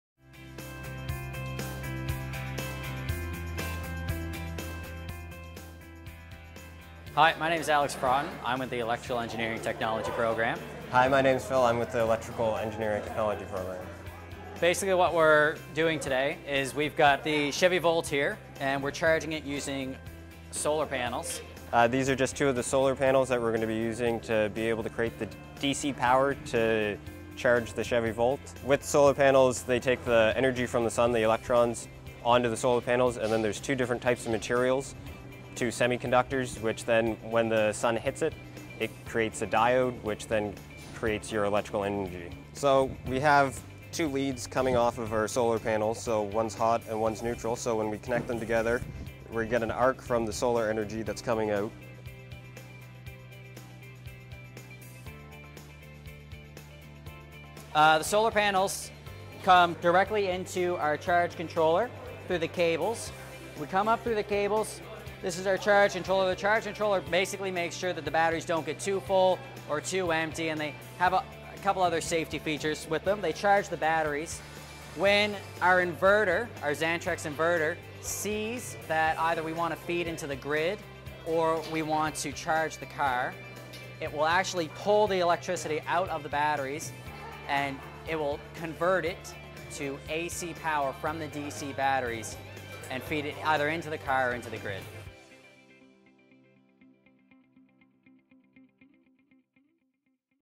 Solar Recharging Station: Chevy Volt
Learn how two NSCC students in the Electrical Engineering Technology program built a solar recharging station for the Chevy Volt.